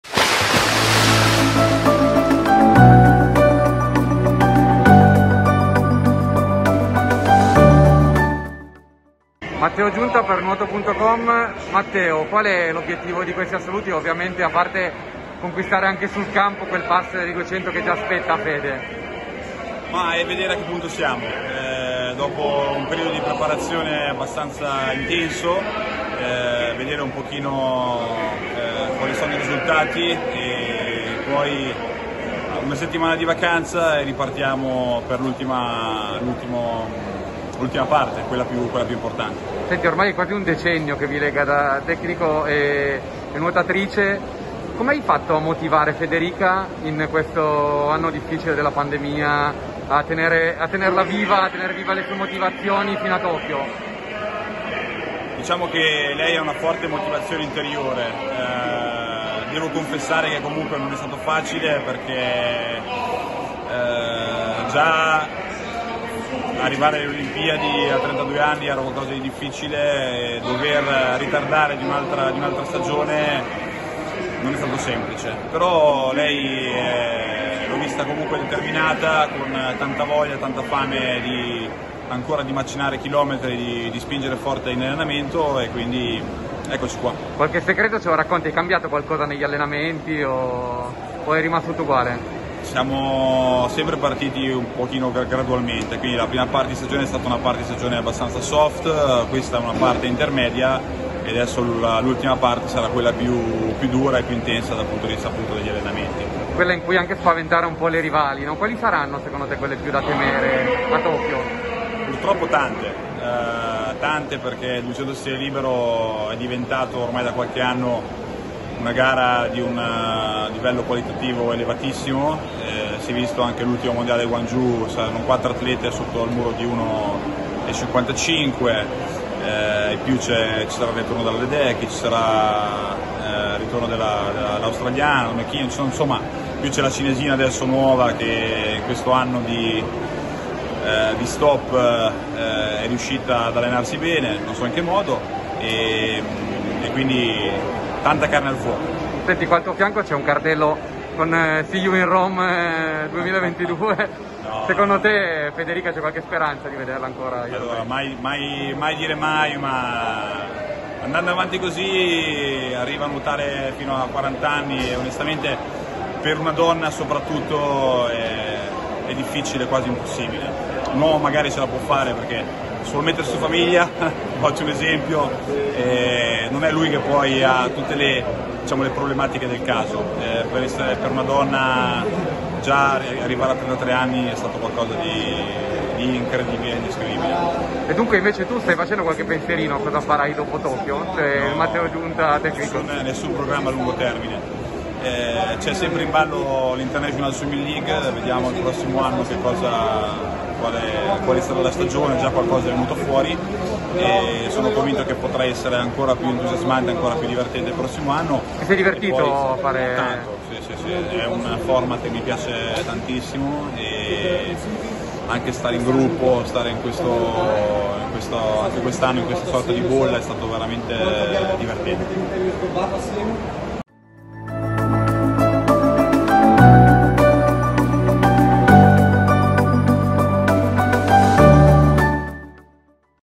Matteo Giunta per Nuoto•com
Matteo Giunta intervistato da Alberto Dolfin, in zona mista ai Campionati Italiani Assoluti FIN UnipolSai.